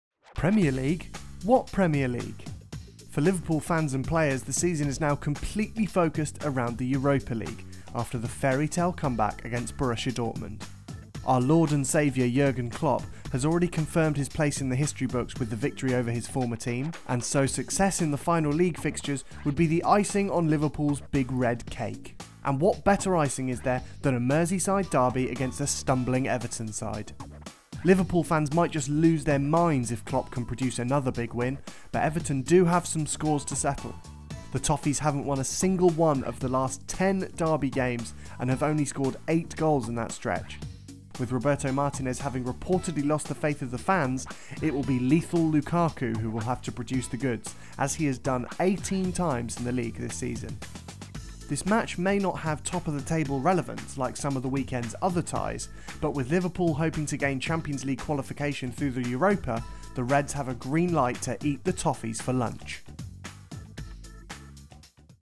Liverpool v Everton - Premier League match preview
A look at the Premier League clash between Liverpool and Everton.